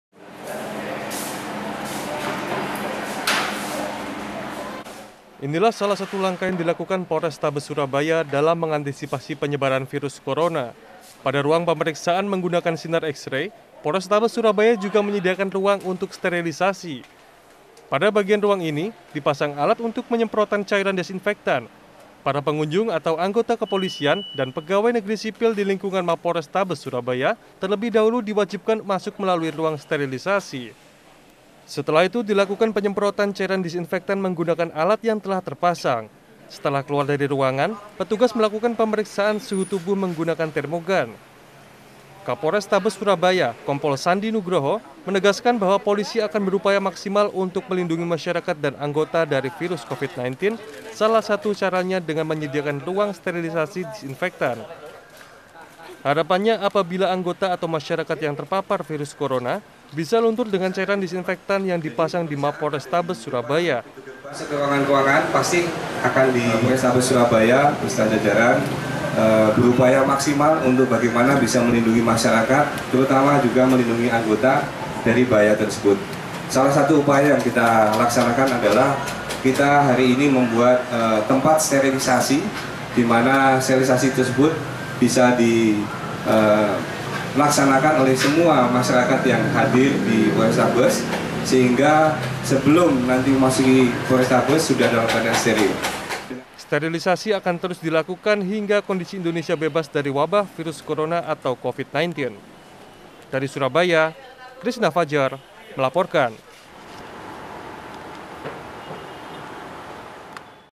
Polisi Siapkan Ruang Sterilisasi Disinfektan
SURABAYA - untuk mengantisipasi penyebaran covid-19 Polrestabes Surabaya melakukan berbagai langkah. satu diantaranya adalah menyediakan tempat sterilisasi terhadap pengunjung atau anggota kepolisian yang hendak masuk ke markas Polrestabes Surabaya.